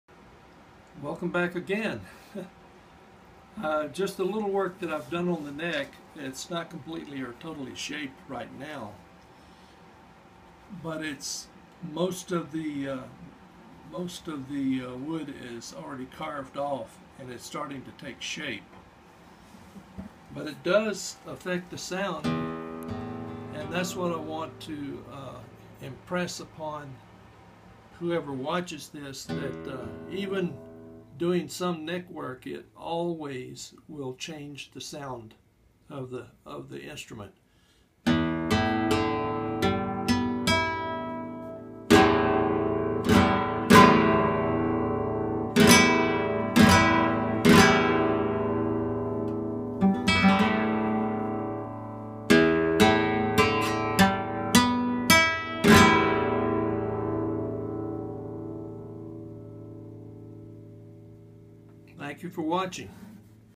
Tone difference after shaping the neck
Tom Blackshear Flamenco / Manuel Reyes Style Replica /Compilation of different designs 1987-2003